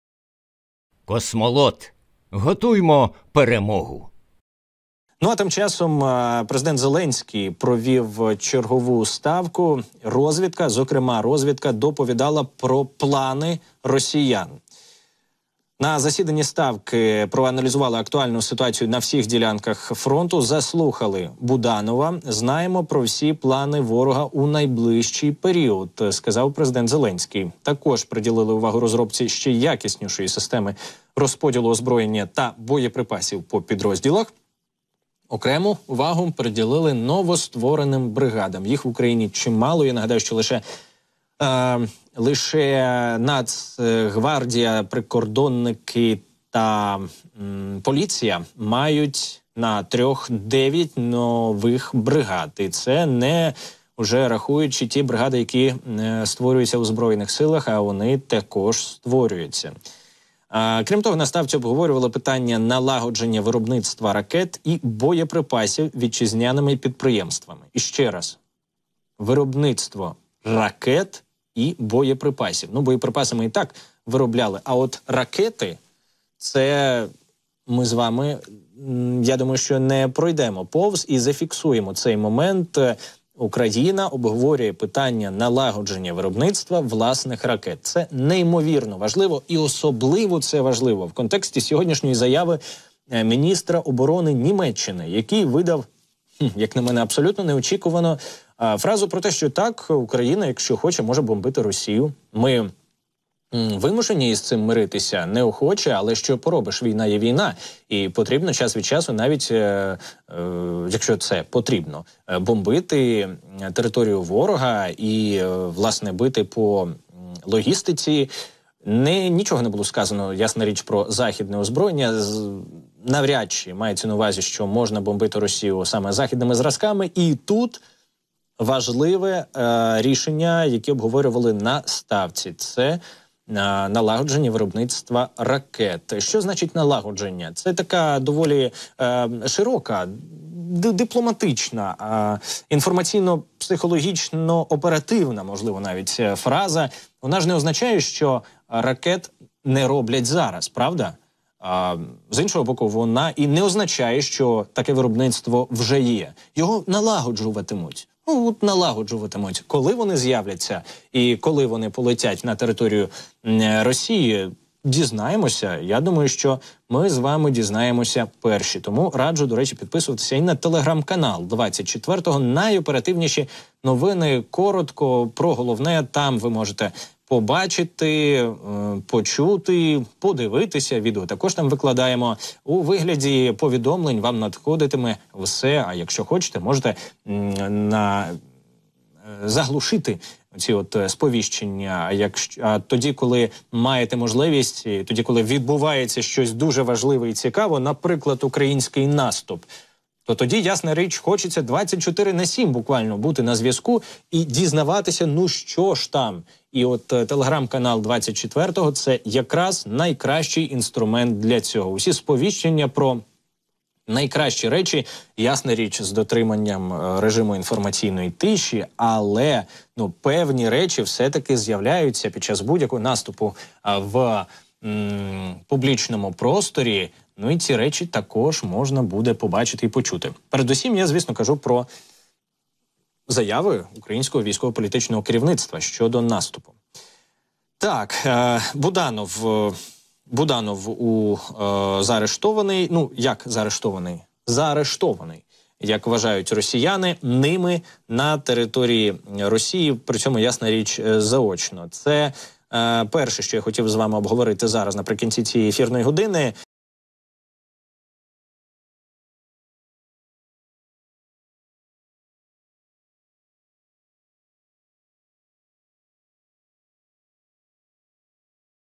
"Знаємо всі плани ворога!" / БУДАНОВ доповів ЗЕЛЕНСЬКОМУ / Деталі
Володимир Зеленський провів засідання Ставки Головнокомандувача. Кирило Буданов доповів, що наша розвідка знає про всі плани ворога у найближчий період.
Засідання Ставки відбулось 21 квітня. Президент України розповів, що на Ставці обговорювали виробництво ракет і боєприпасів нашими підприємствами.